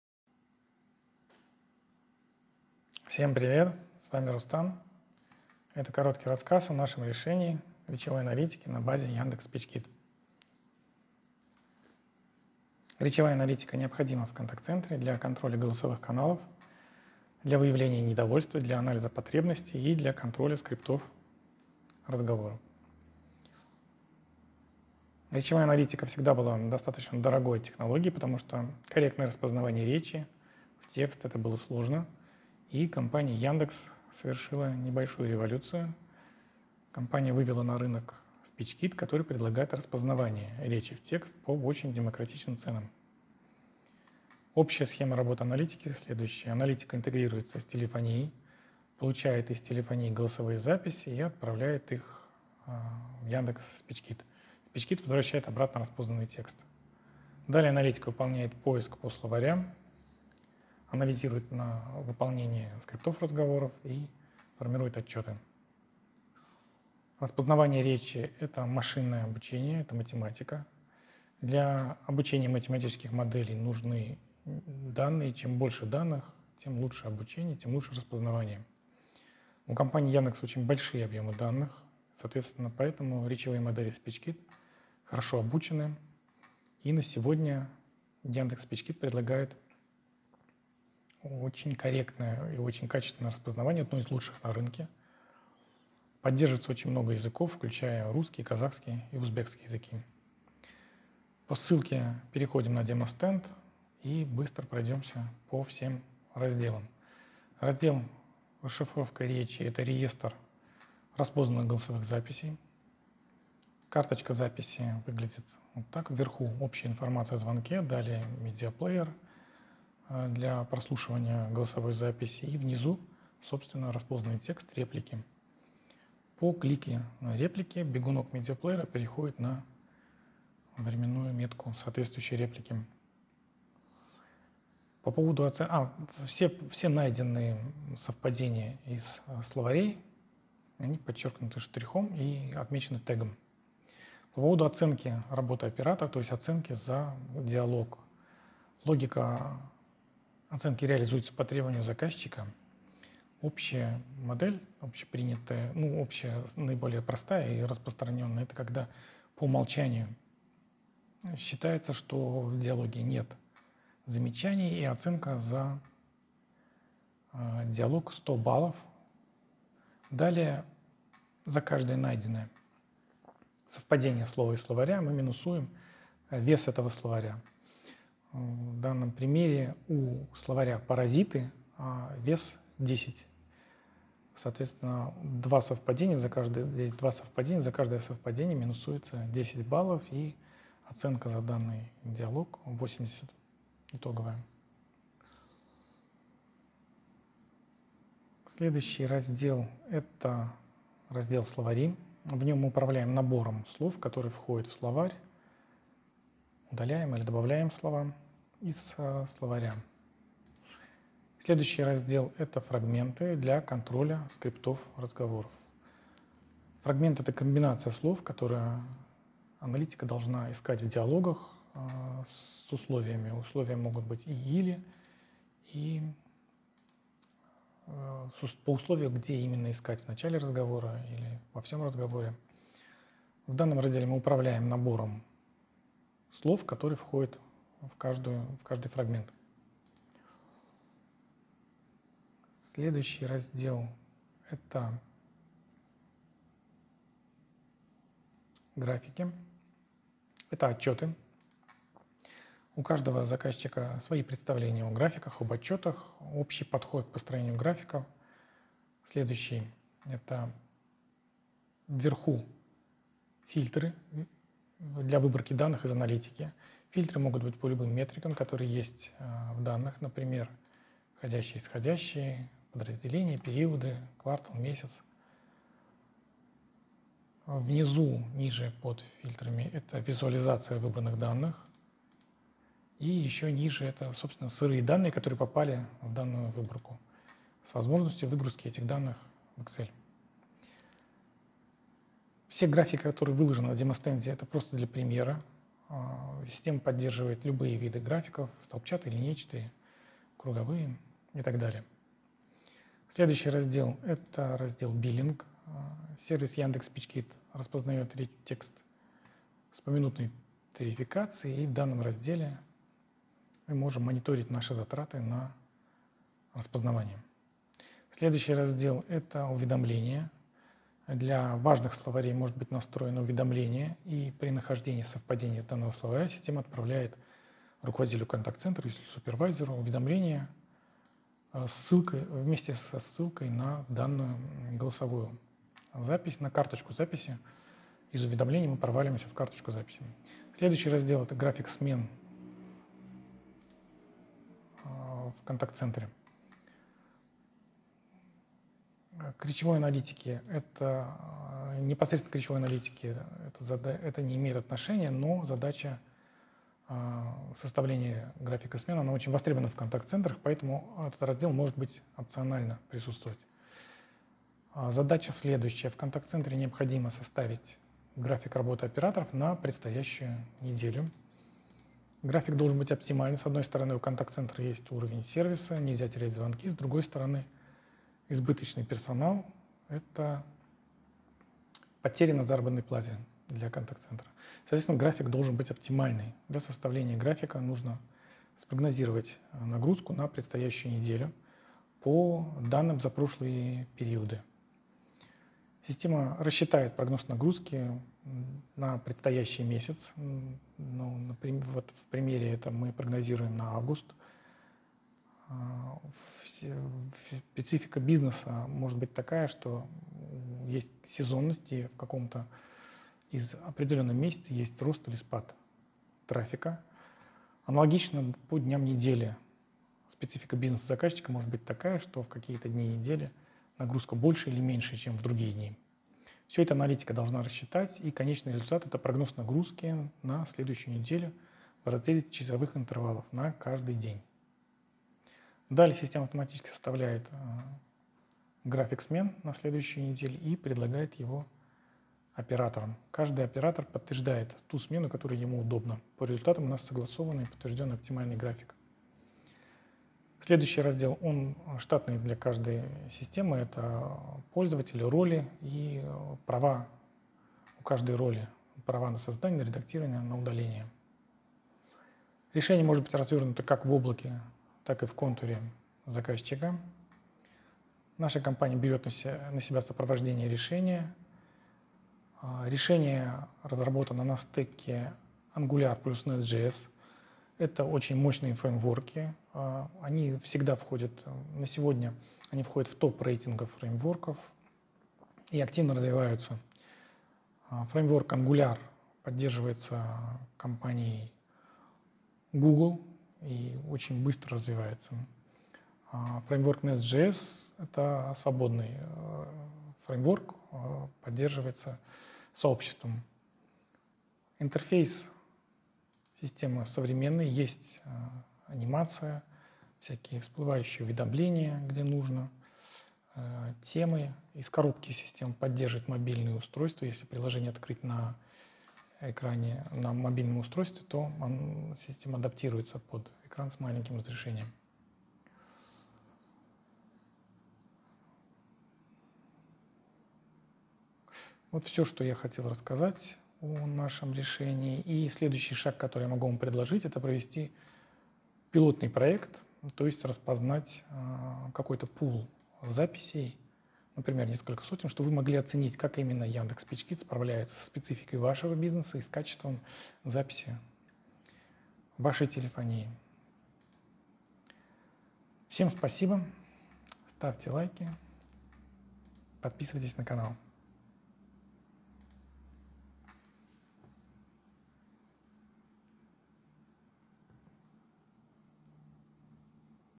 Речевая аналитика на базе Yandex SpeechKit (English subs)
Yandex SpeechKit - распознавание речи с высоким качеством. Решение на базе Open Source библиотек, без покупки лицензий.